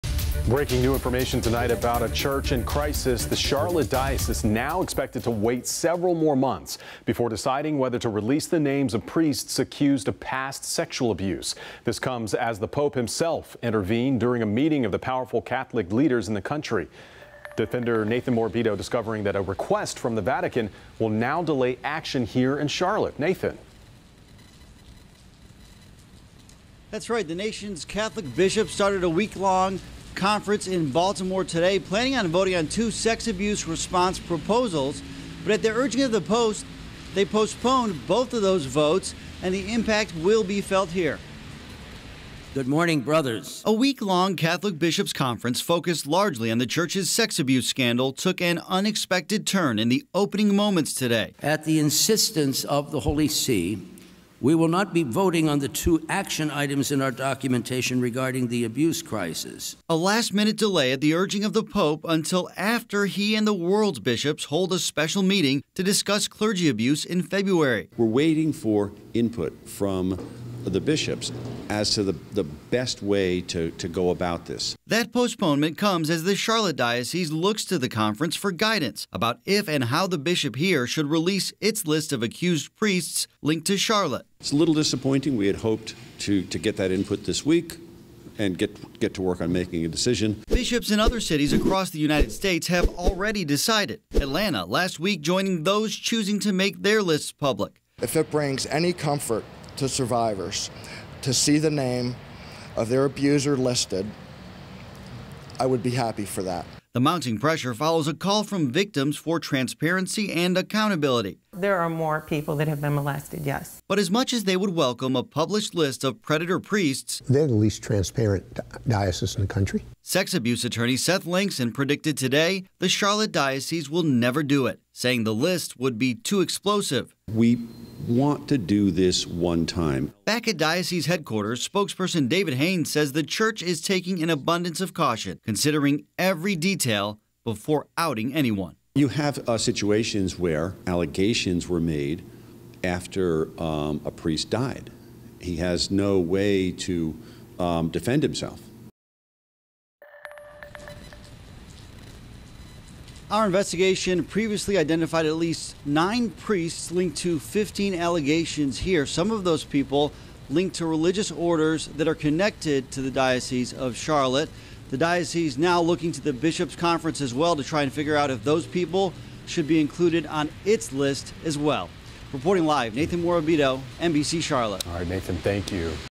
Charlotte Catholic church delays release of names linked to sex abuse
The Diocese of Charlotte is pushing back when they could release the names of priests linked to sex abuse by several months after a decree from Pope Francis.